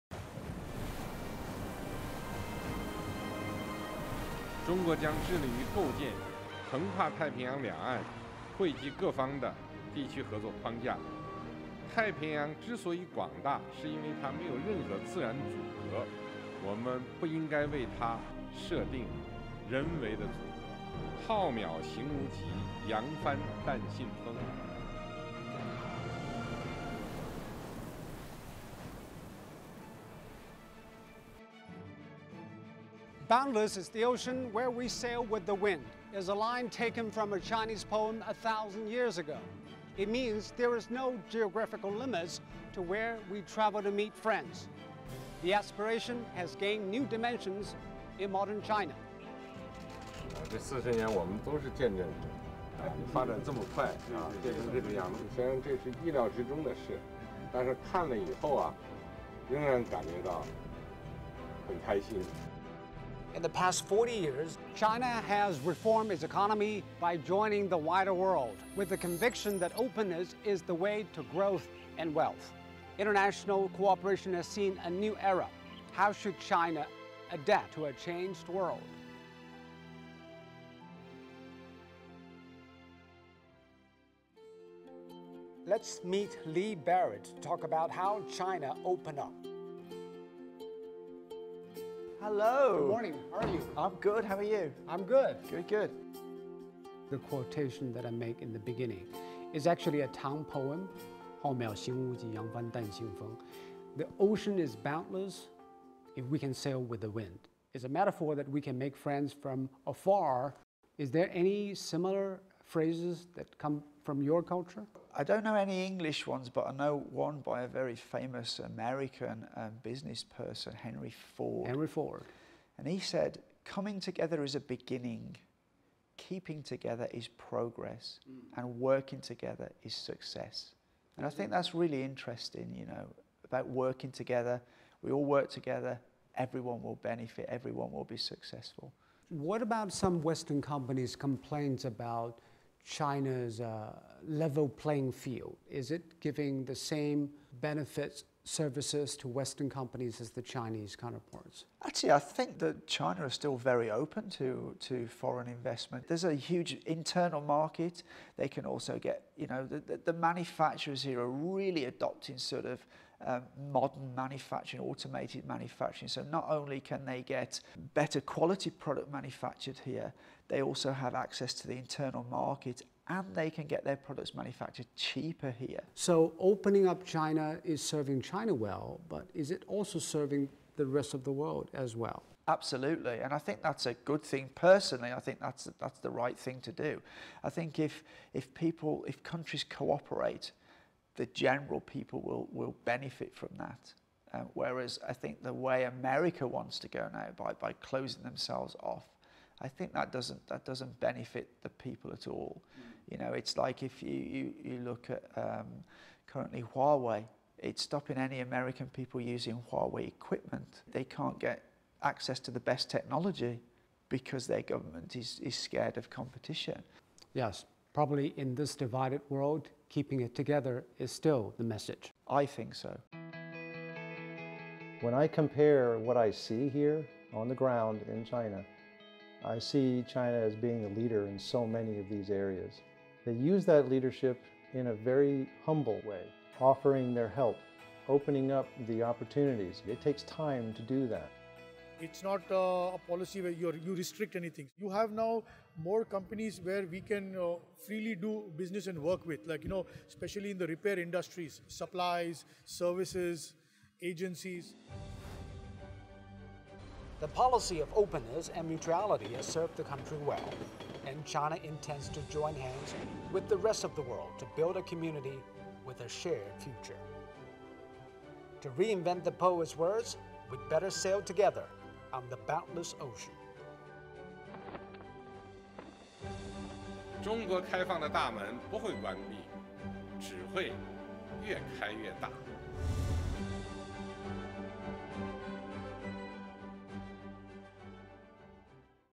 China to deepen reform, opening-up, expand cooperation in new era
With the conviction that openness is the way to growth and wealth, Chinese President Xi Jinping has called on China and the rest of the world to expand cooperation and build a community with a shared future for mankind.
While attending the APEC CEO Summit in Bali, Indonesia back in November 2013, the Chinese president quoted a time-honored saying to express the country's firm determination to reform and open up its economy and enhance cooperation with the rest of the world.
"China will commit itself to building a cross-Pacific regional cooperation framework that benefits all parties. The vast Pacific is free of natural barriers, and we should not erect any man-made ones. 'Boundless is the ocean where we sail with the wind,'" Xi said, quoting a line from a Chinese poem written 1,000 years ago.
The ancient saying stresses there are no geographical limits to where people can travel to meet friends. This aspiration has gained new dimensions in modern China.